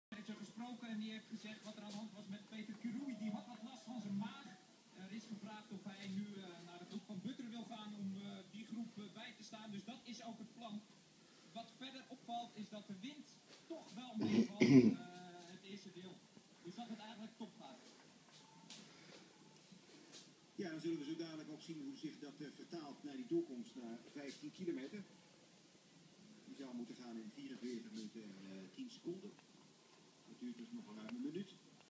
Idejo man Richard de Jong achterop bij Andre Goud en op de
Idejo man fiets al jaren mee met de Marathon in Rotterdam. Dit jaar loopt de IDEJO man op krukken. Dus dan maar achterop de tandem en voor het goede doel Mercy Ships